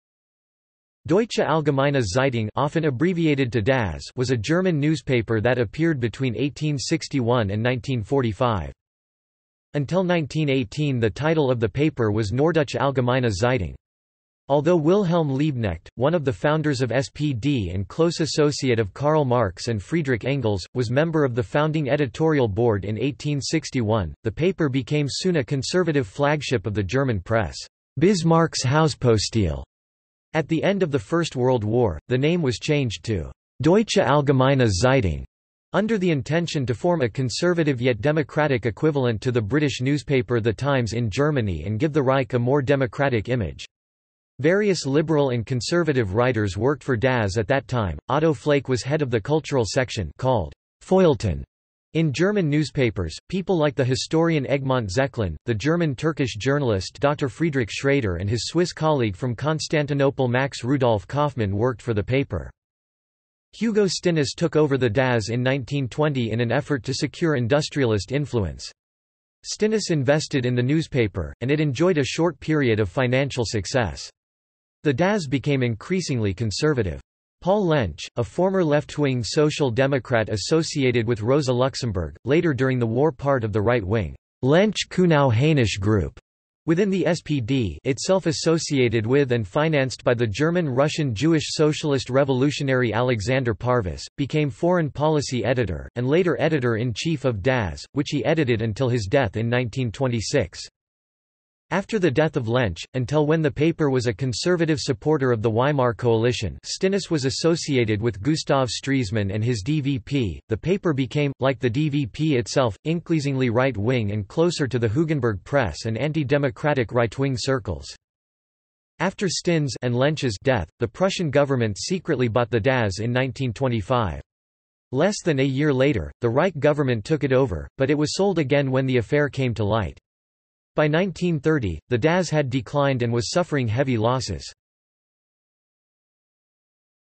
Deutsche Allgemeine Zeitung | Wikipedia audio article
This is an audio version of the Wikipedia Article:
Deutsche Allgemeine Zeitung

- Socrates

SUMMARY
Deutsche Allgemeine Zeitung (often abbreviated to DAZ) was a German newspaper that appeared between 1861 and 1945.
Until 1918 the title of the paper was Norddeutsche Allgemeine Zeitung. Although Wilhelm Liebknecht, one of the founders of SPD and close associate of Karl Marx and Friedrich Engels, was member of the founding editorial board in 1861, the paper became soon a conservative flagship of the German press ("Bismarcks Hauspostille"). At the end of the First World War, the name was changed to "Deutsche Allgemeine Zeitung", under the intention to form a conservative yet democratic equivalent to the British newspaper The Times in Germany and give the Reich a more democratic image. Various liberal and conservative writers worked for DAZ at that time, Otto Flake was head of the Cultural Section ( called "Feuilleton" in German newspapers ), people like the historian Egmont Zechlin, the German-Turkish journalist Dr. Friedrich Schrader and his Swiss colleague from Constantinople Max Rudolf Kaufmann worked for the paper.
Hugo Stinnes took over the DAZ in 1920 in an effort to secure industrialist influence.  Stinnes invested in the newspaper, and it enjoyed a short period of financial success. The DAZ became increasingly conservative. Paul Lensch, a former left-wing Social Democrat associated with Rosa Luxemburg, later during the war part of the right wing "Lensch-Cunow-Haenisch-Gruppe" within the SPD (itself associated with and financed by the German-Russian-Jewish socialist revolutionary Alexander Parvus), became foreign policy editor, and later editor in chief of DAZ, which he edited until his death in 1926.
After the death of Lensch, until when the paper was a conservative supporter of the Weimar coalition (Stinnes was associated with Gustav Stresemann and his DVP), the paper became, like the DVP itself, incleasingly right wing and closer to the Hugenberg press and anti-democratic right-wing circles.
After Stinnes' (and Lensch's) death, the Prussian government secretly bought the DAZ in 1925.  Less than a year later, the Reich government took it over, but it was sold again when the affair came to light.By 1930, the DAZ had declined and was suffering heavy losses.